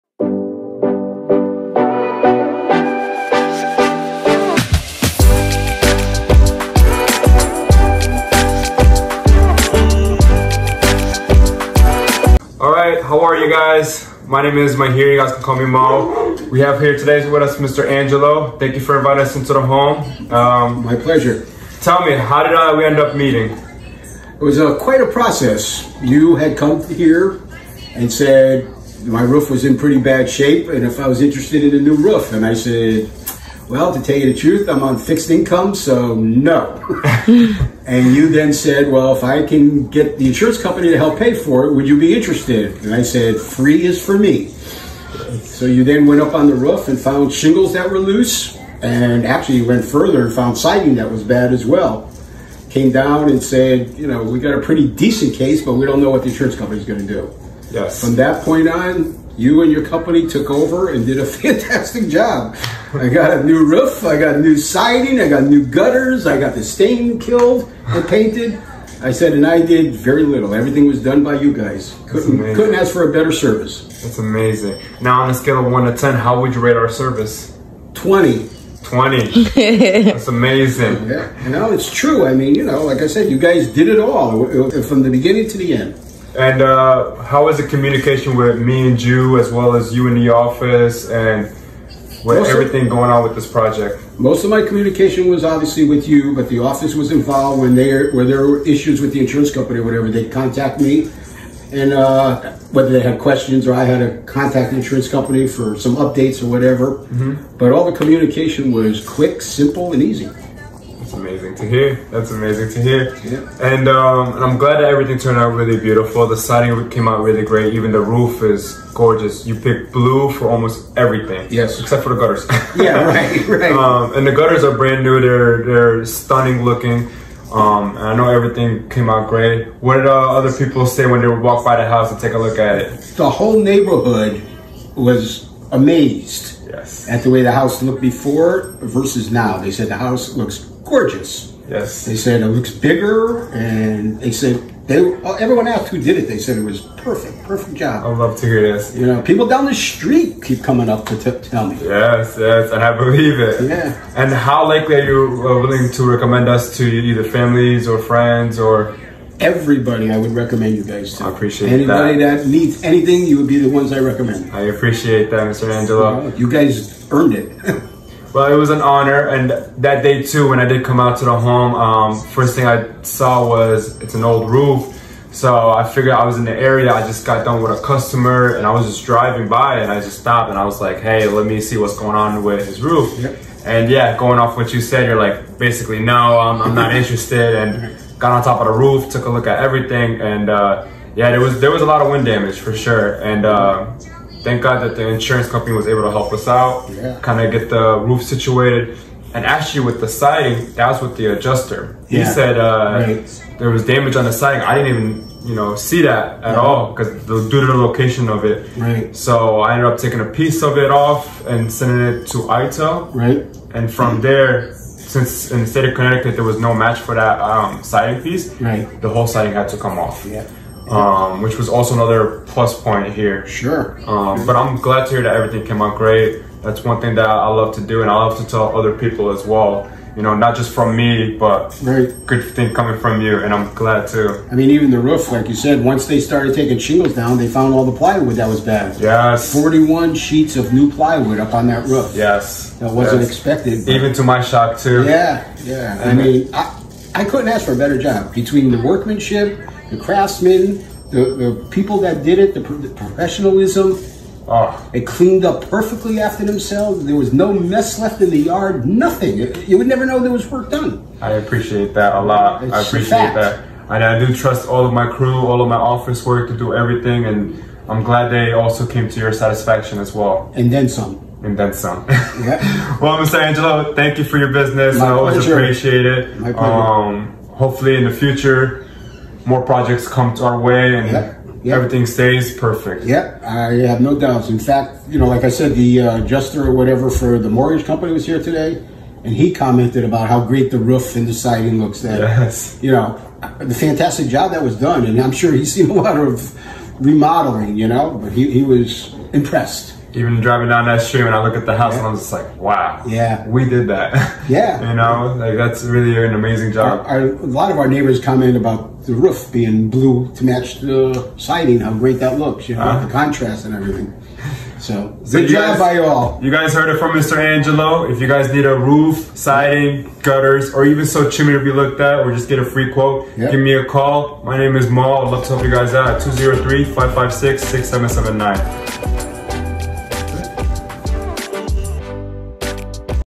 Homeowner Testimony with Mahir
Homeowner testimony by Mr Angelo and Mahir for our project of roof and siding replacement.

This videos discuss the the honestly review from the homeowner in regards to our professionalism, the process and the final results!

If you are considering a replacing a roof and or siding replacement and want to hear firsthand account of working with a dedicated professionalism. This video is for you and Our company is for you!